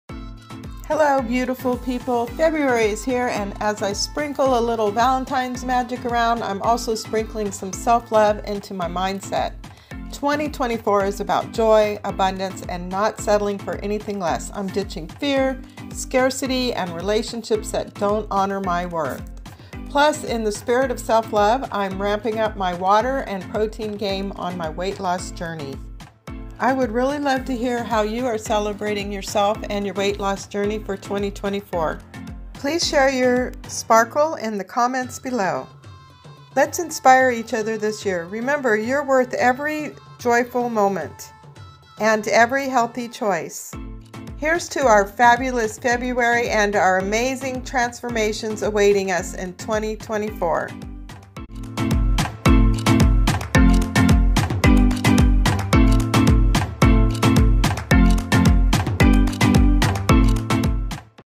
Elevate your Mindset for 2024
Enjoy this video about thriving and embracing confidence and owning your worth with a mindset makeover for 2024
Thrive , Shed Fear, and Embrace your worth . Join me on my weightloss journey let's make this the best year ever.